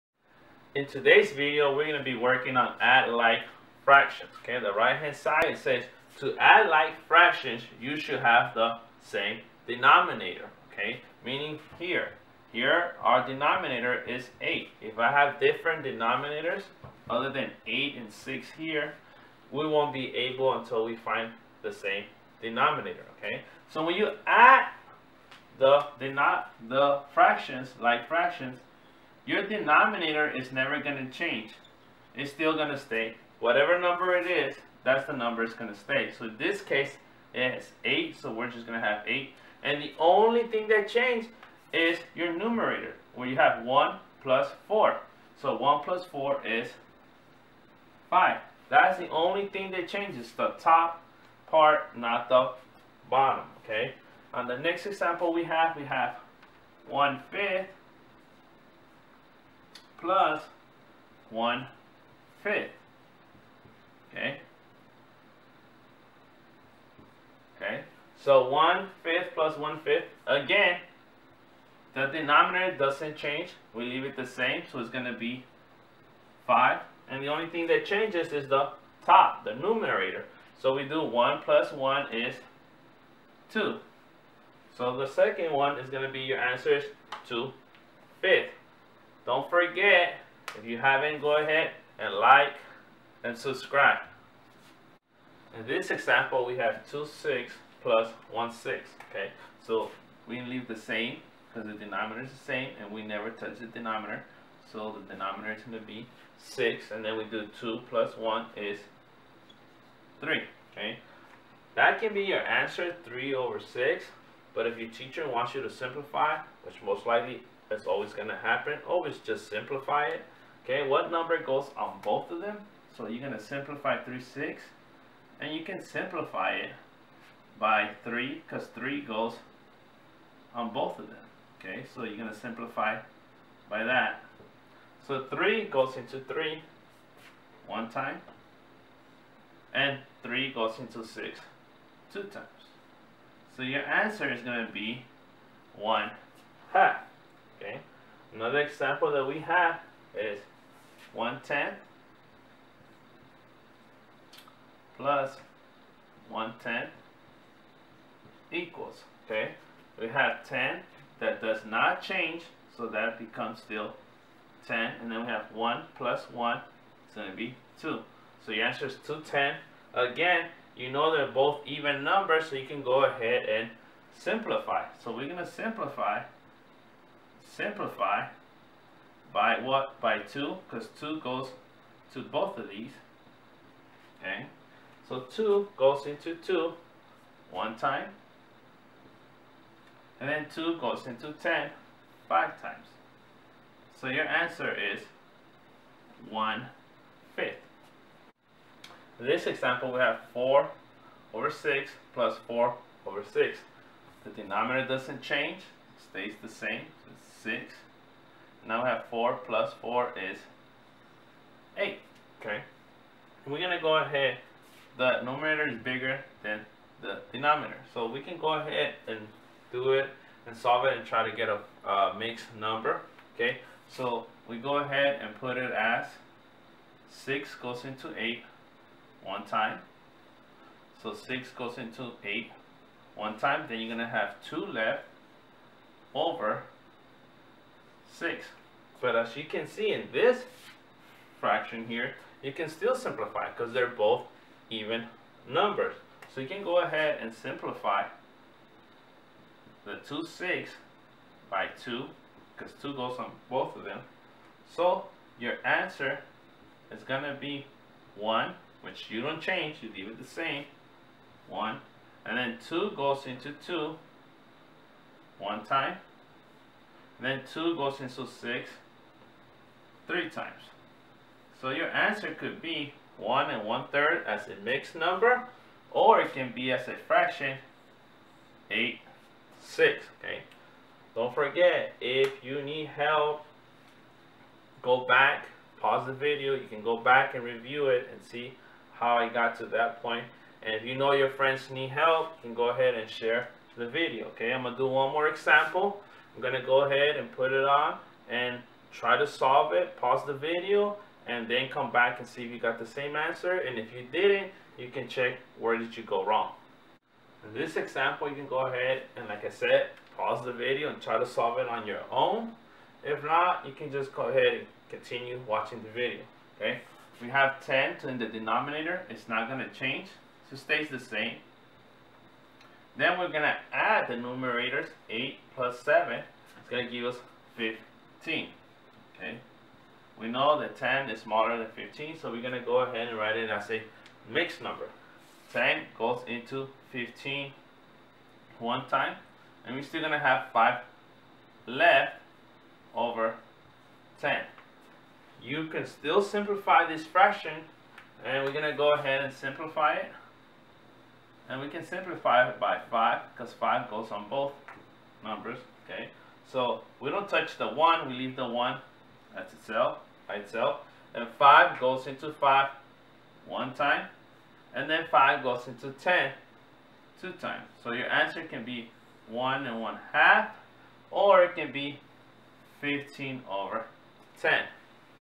Add like fractions
Teaching you how to add like fractions.